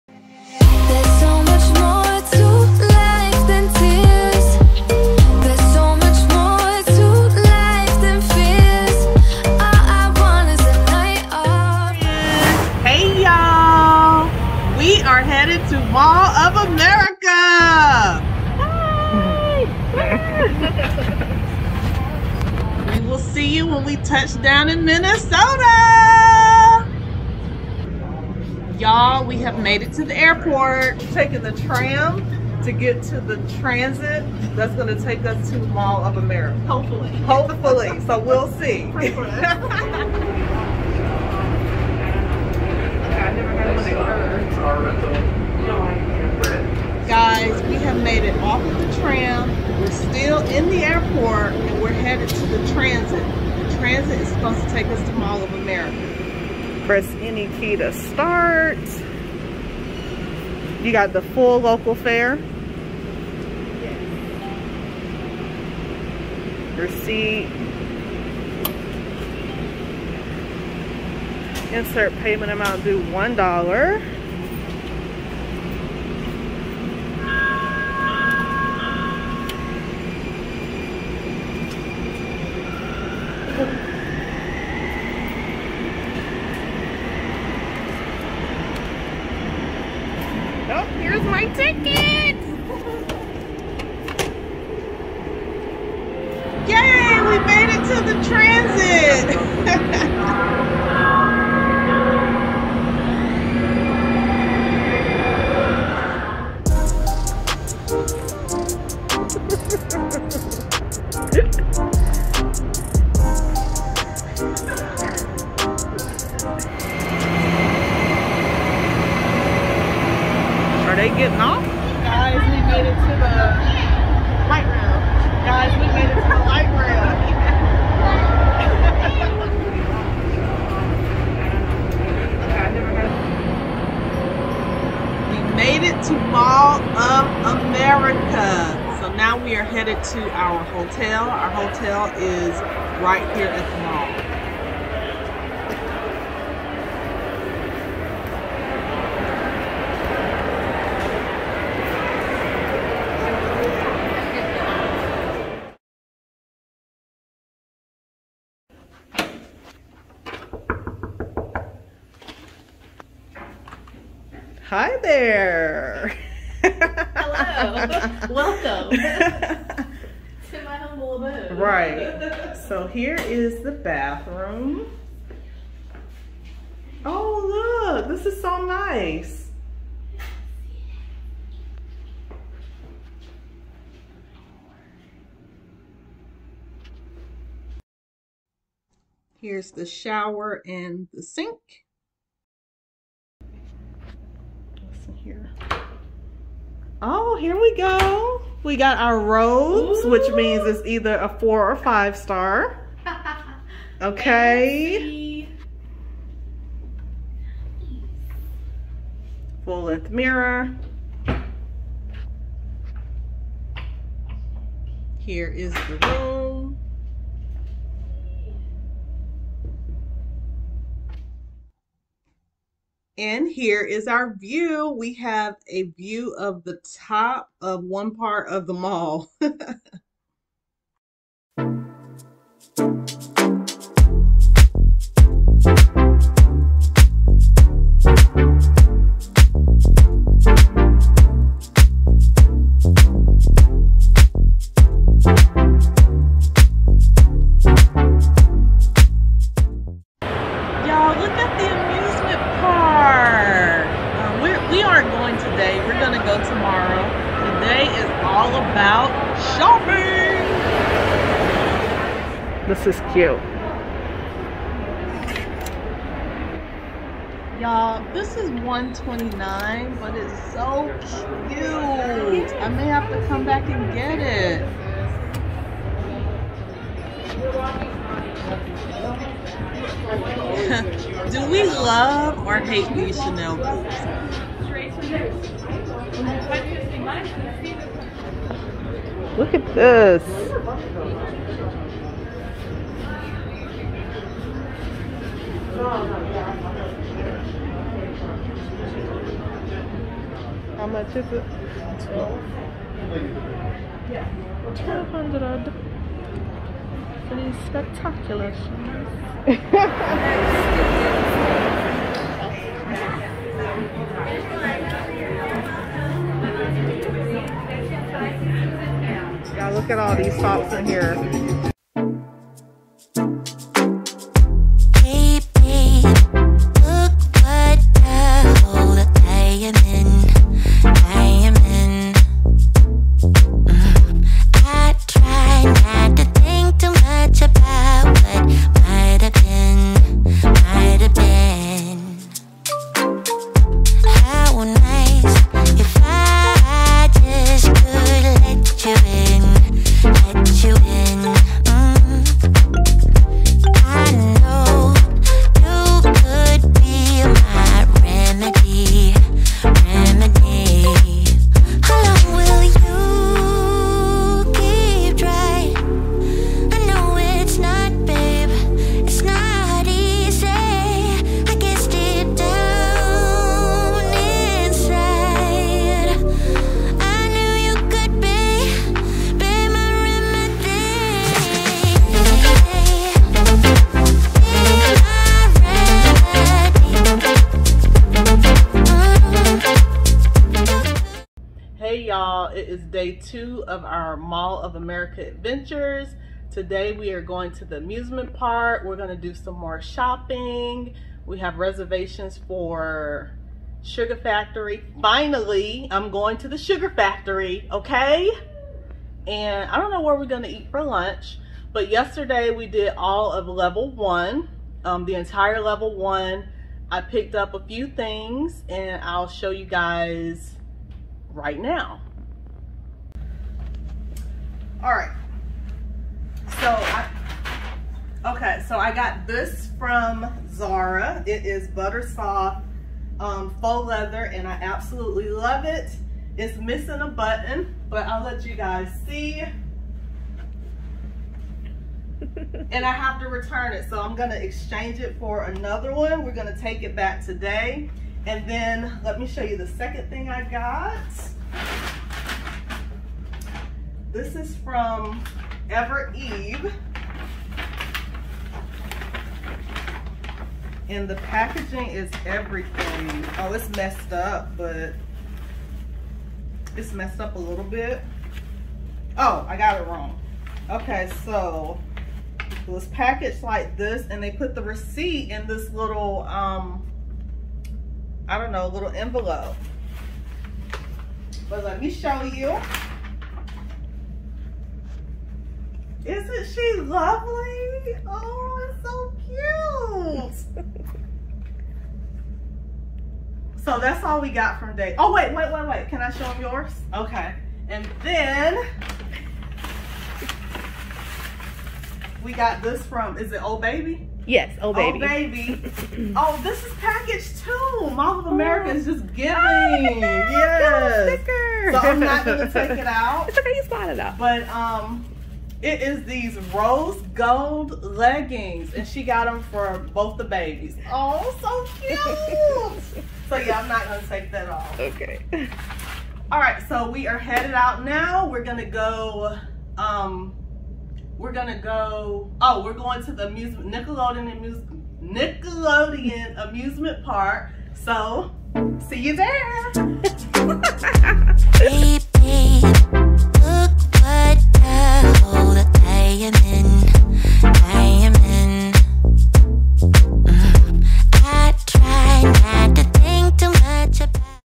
Mall of America Travel Vlog | JW Marriott Minneapolis Room Tour | Shopping Spree | Nordstrom | Zara
I finally made it to Mall of America!!!  Come with me as we try to figure out how to get to Mall of America from the airport lol and shop, shop...shop.  Shop Eat Repeat! Room Tour of JW Marriott Minneapolis Mall of America